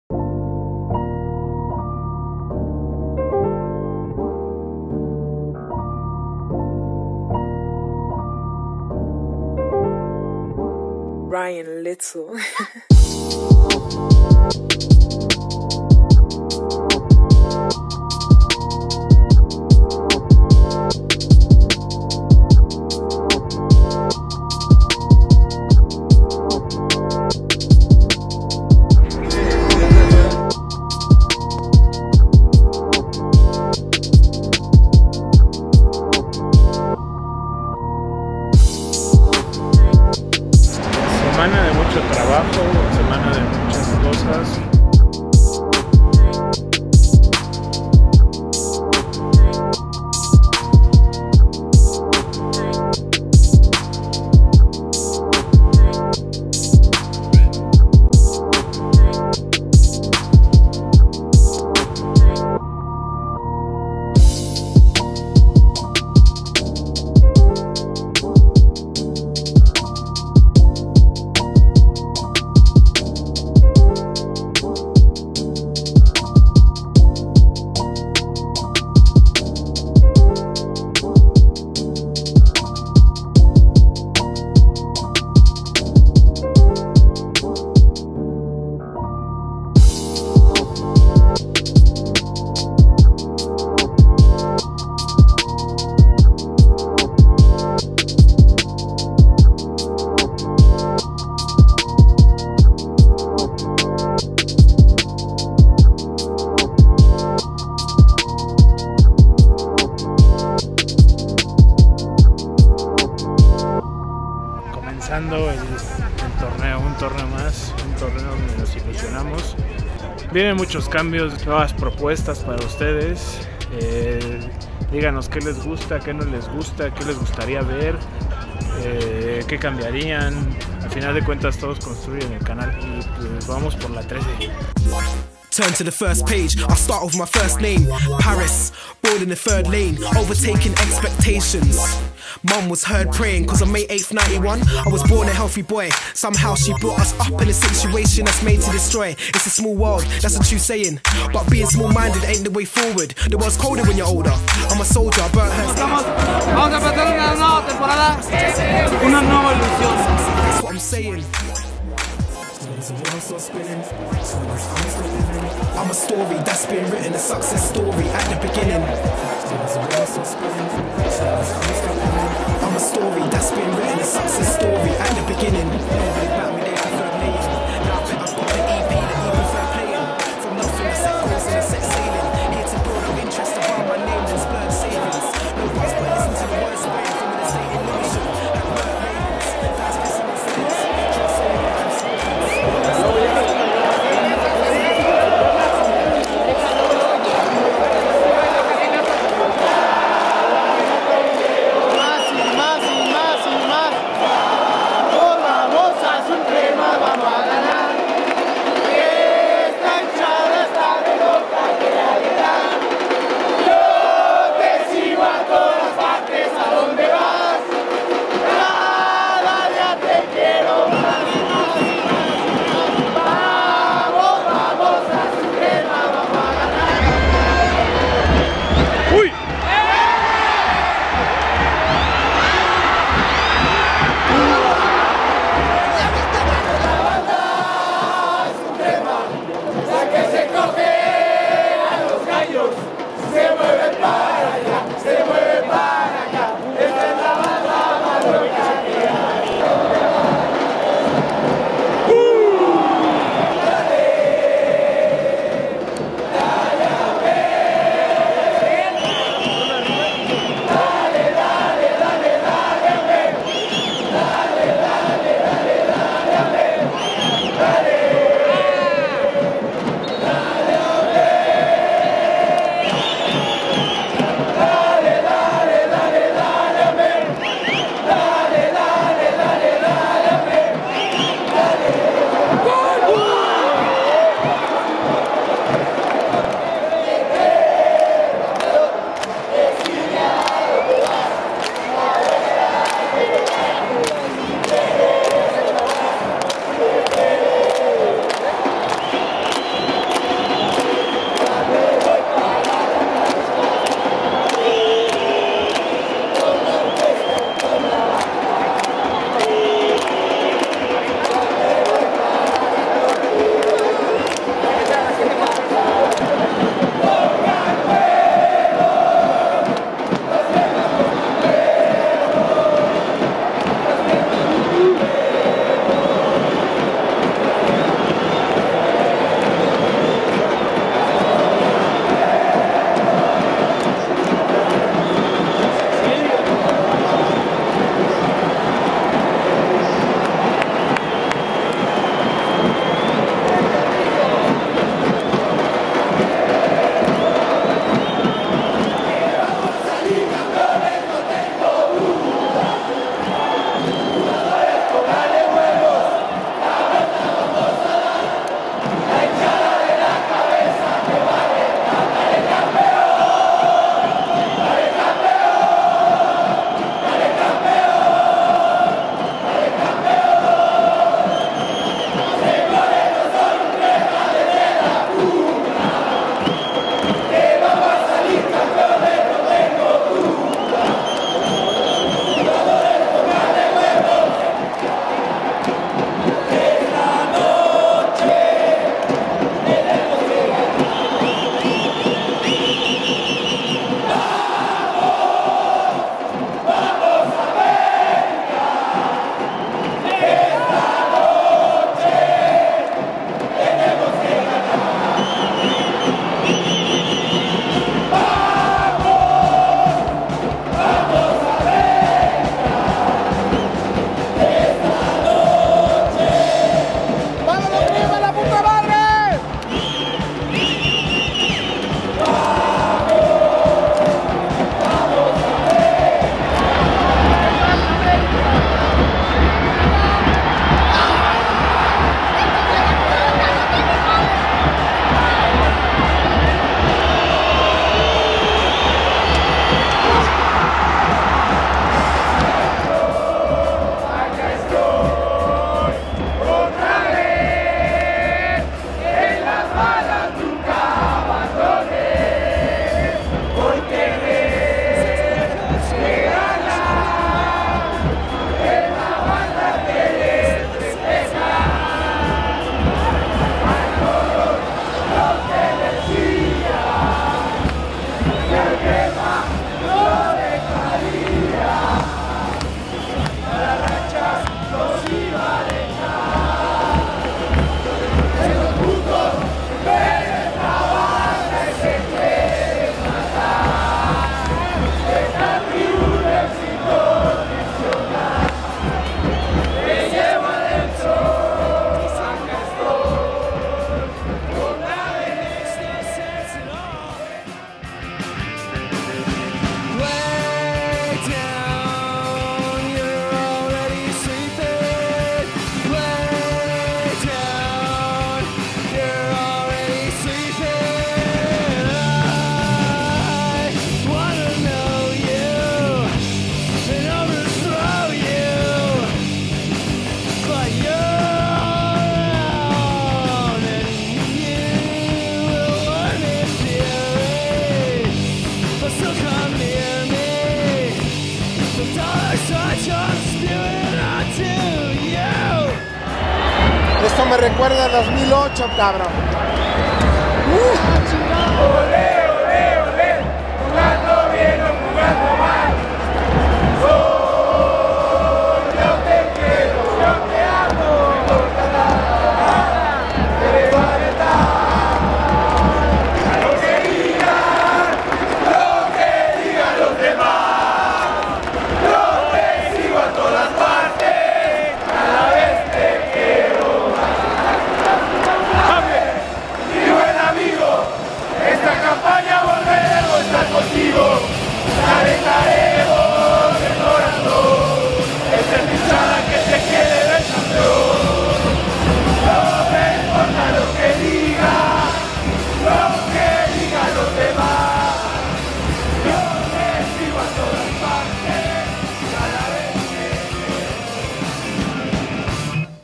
Inicio complicado de torneo - Vlog #41 - Del Crema Soy
America vs Queretaro, asistimos como cada 15 dias al estadio azteca a presenciar un partido mas del club america que recibia a gallos de Queretaro en su visita al estadio azteca.
America deberia de llevarse la victoria en su presentación en el torneo Apertura 2017 de la liga Bancomer mx pero america perdio contra Querétaro; america 0 vs Querétaro 1
el resumen del america vs Querétaro es que america no tuvo llegadas y Queretaro con un penal a favor se levó la victoria en el estadio azteca por la minima diferencia uno a cero